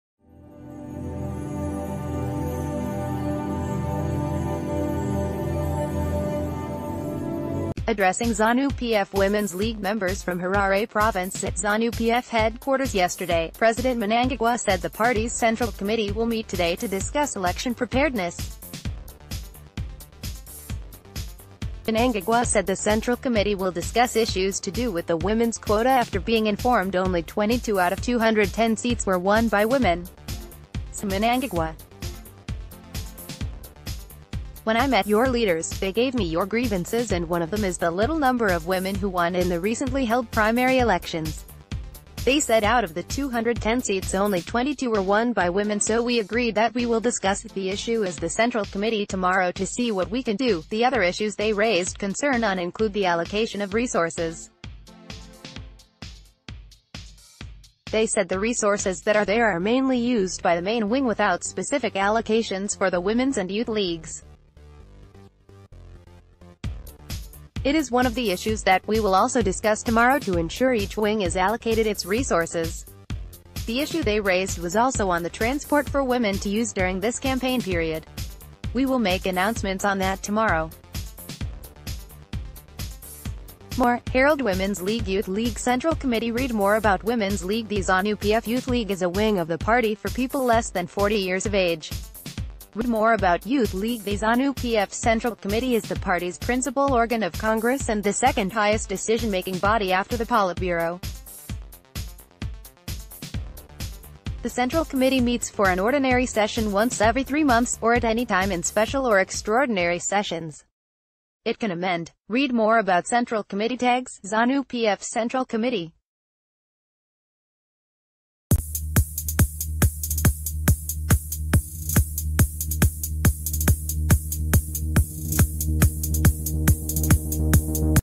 Breaking News | Zanu-PF Central Committee Meets To Discuss Election Preparedness ⋆ Pindula News
Addressing Zanu-PF women’s league members from Harare province at Zanu-PF headquarters yesterday, President Mnangagwa said the party’s central committee will meet today to discuss election preparedness. Mnangagwa said the Central Committee will discuss issues to do with the women’s quota after being informed only 22 out of 210 seats were won by women. Said Mnangagwa. When I met your leaders, they gave me your grievances and one of them is the little number of women who won in the recently held p...